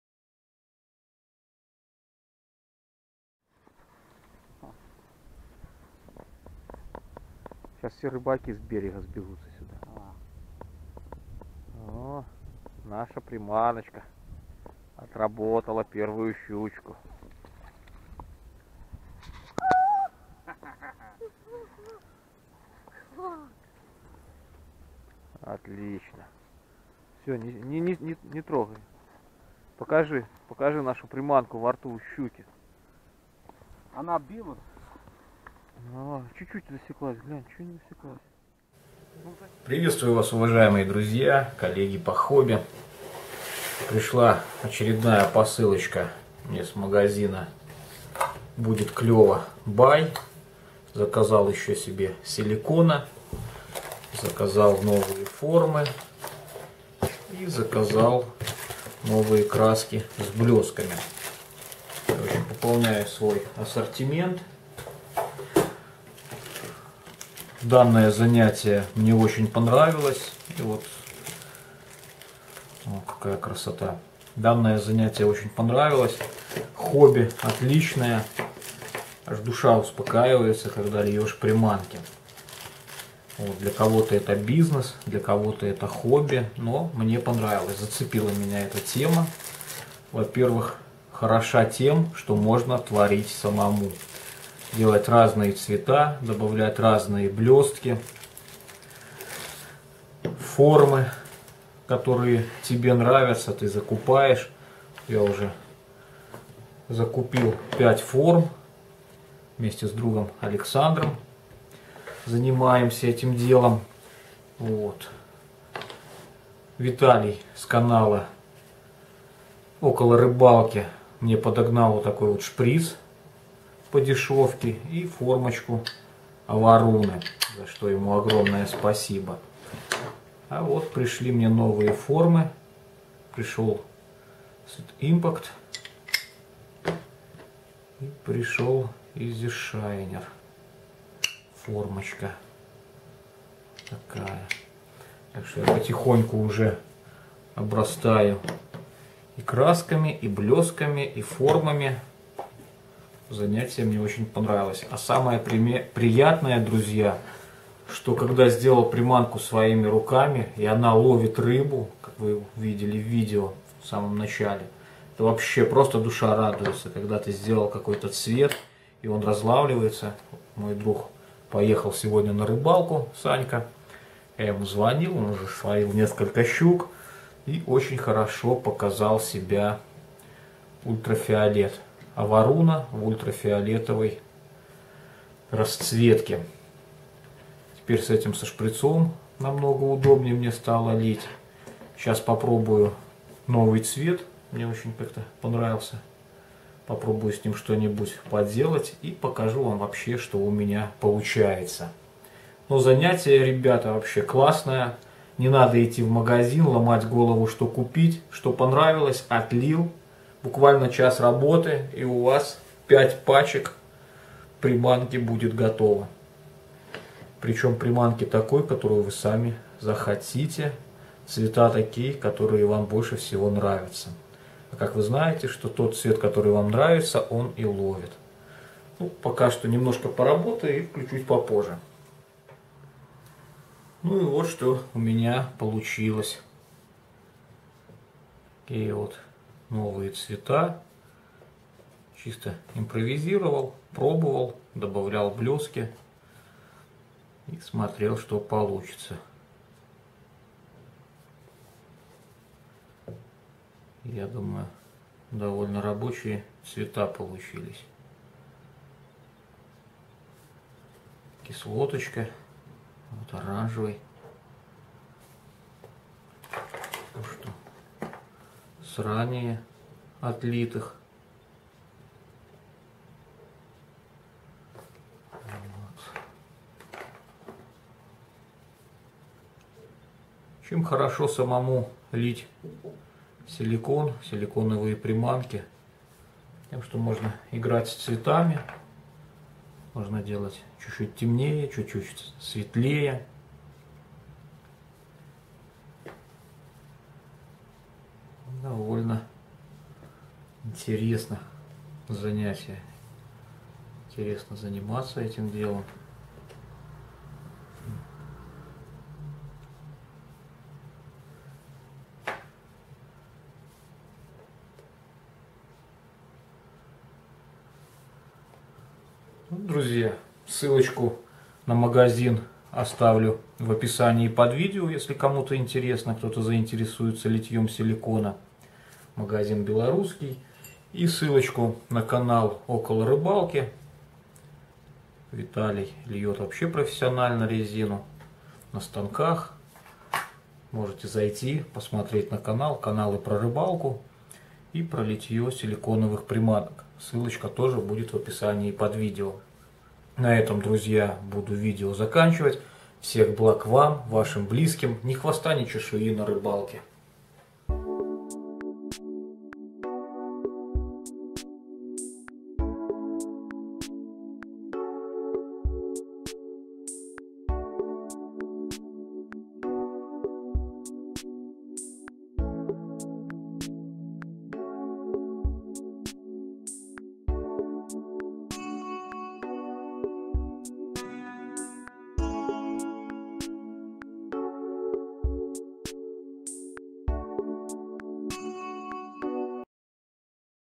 Хобби или бизнес? Силиконовые приманки своими руками. Пополнение ассортимента.
Пополняю ассортимент форм, красок и блёсток! Разлавливаем изготовленные приманки! Работаем с разными цветами! Прикольное это дело лить силиконовые приманки!